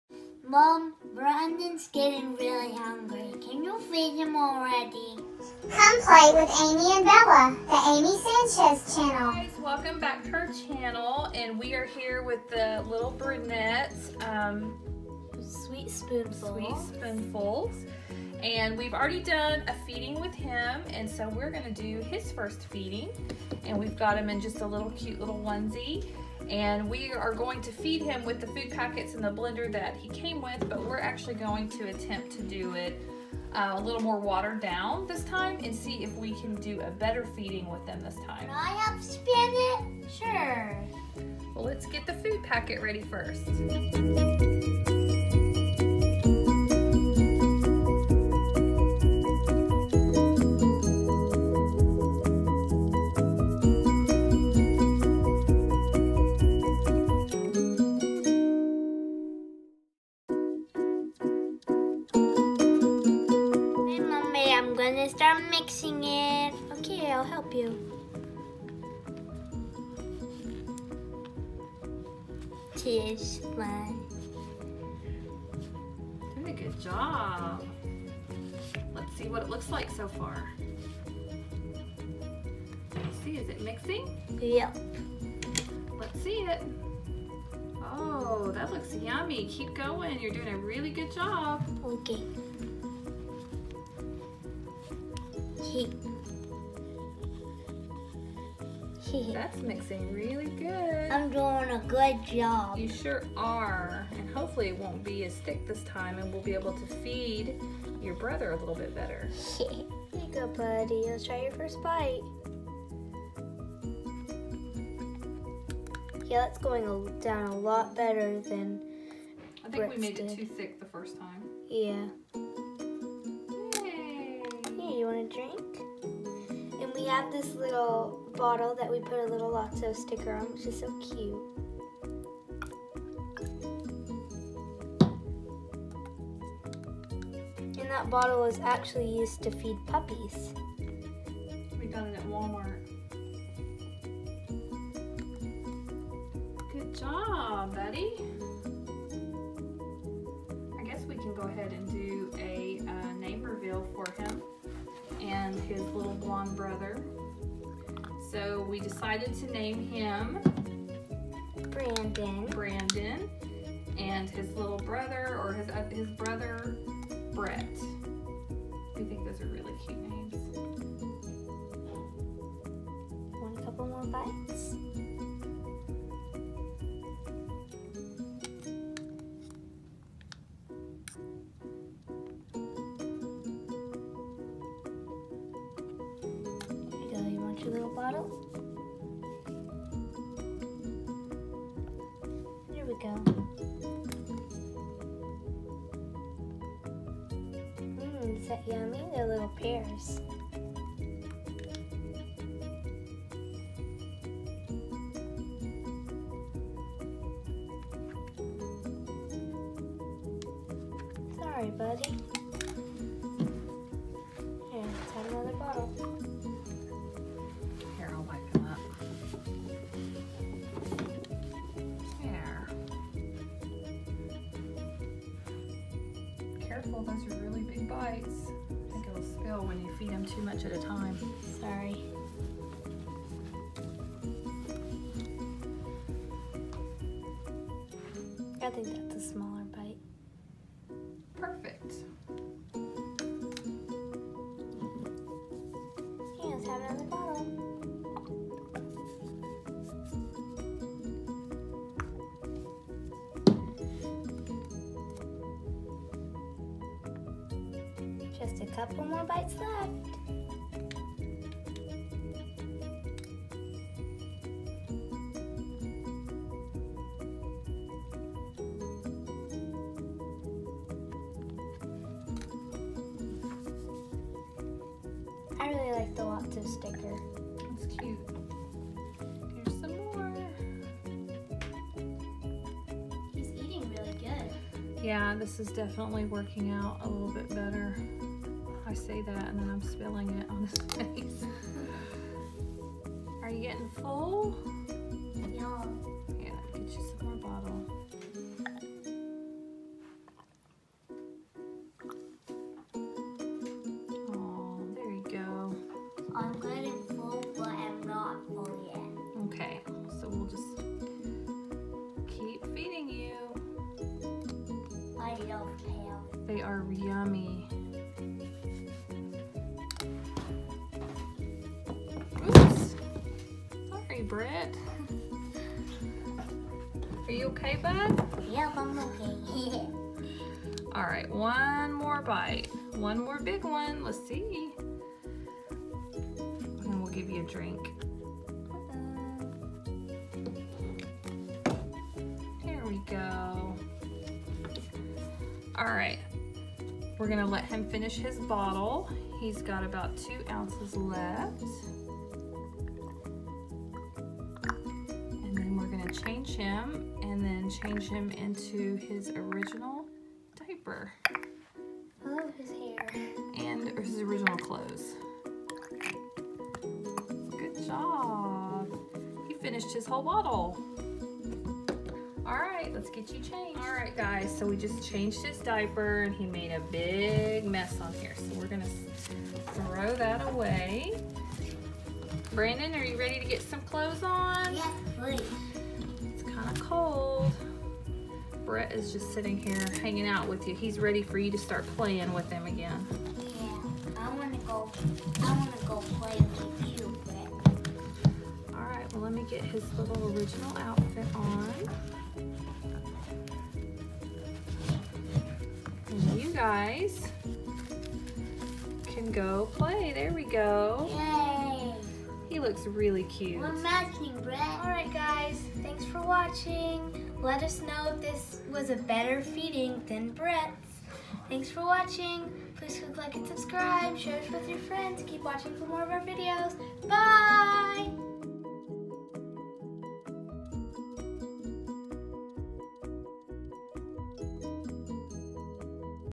BABY ALIVE SWEET SPOONFULS brunette babies first feeding and brother helps
Baby Alive sweet spoonfuls we try feeding our other new baby and see if this feeding will go better! What do you guys think!?